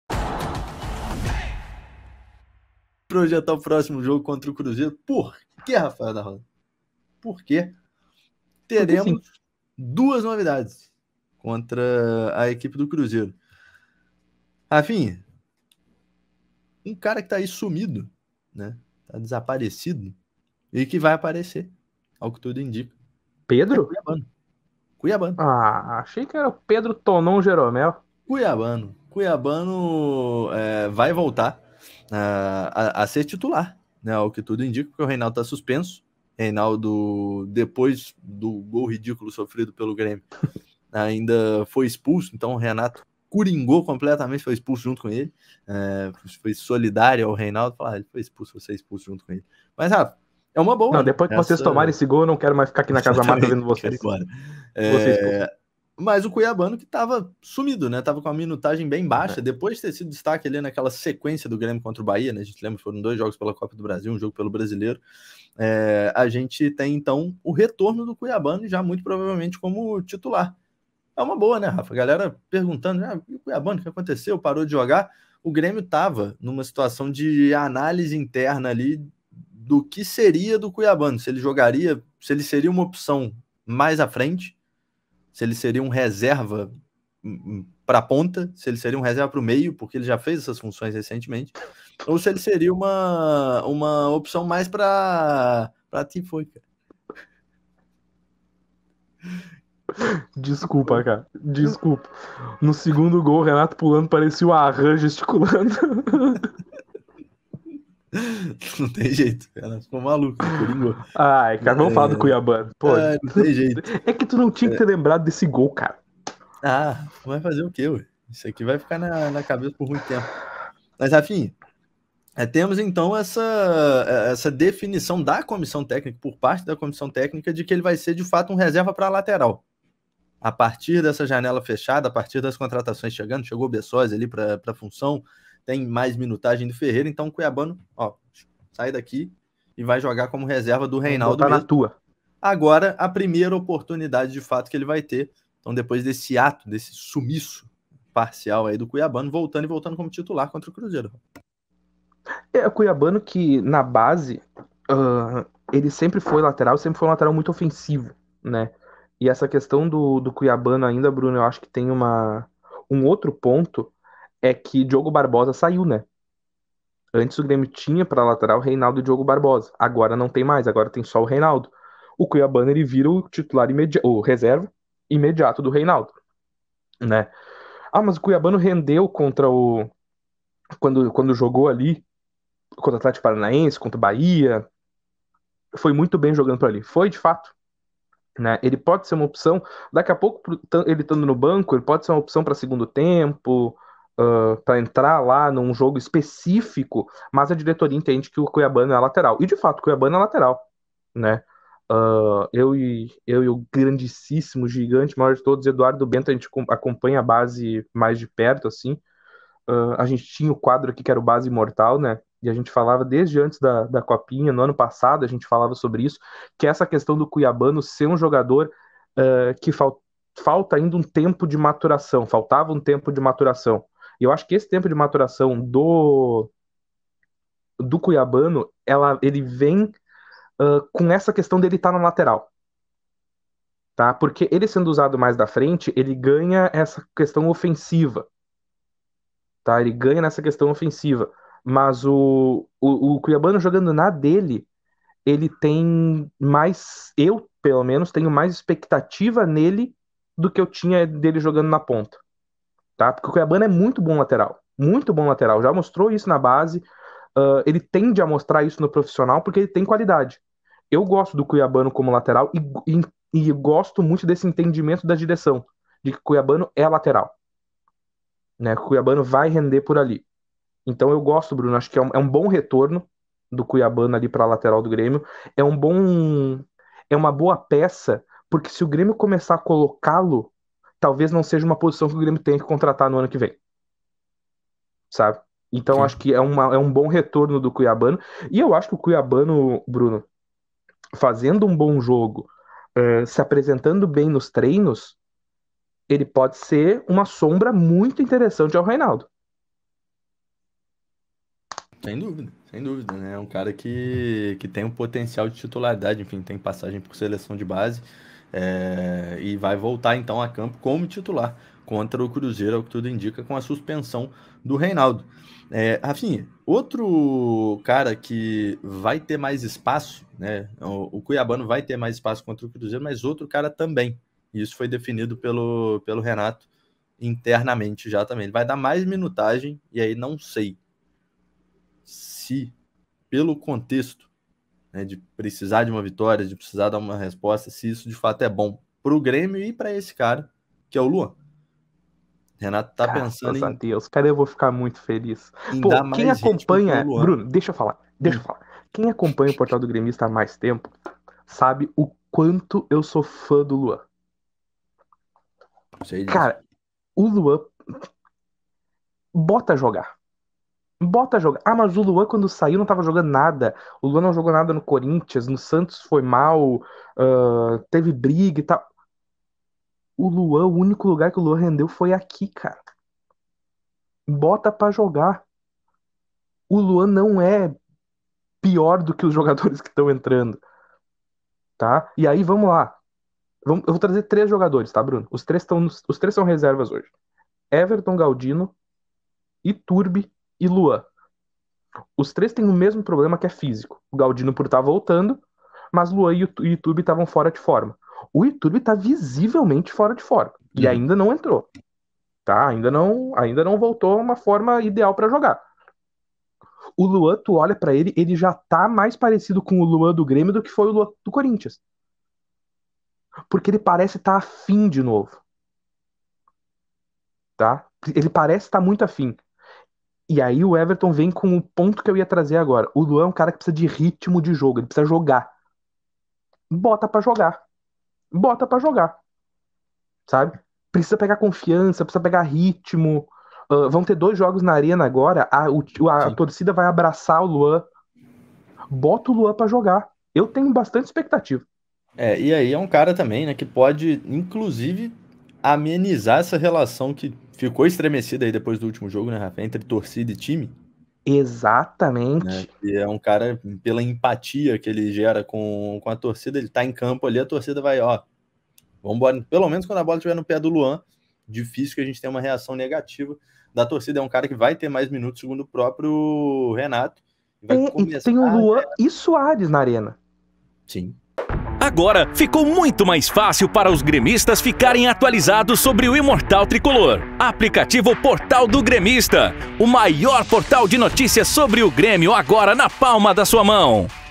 RENATO FAZ DUAS MUDANÇAS NO TIME E JOGADOR SERÁ TITULAR APÓS MUITO TEMPO FORA
Renato Portaluppi prevê mais uma mudança importante no time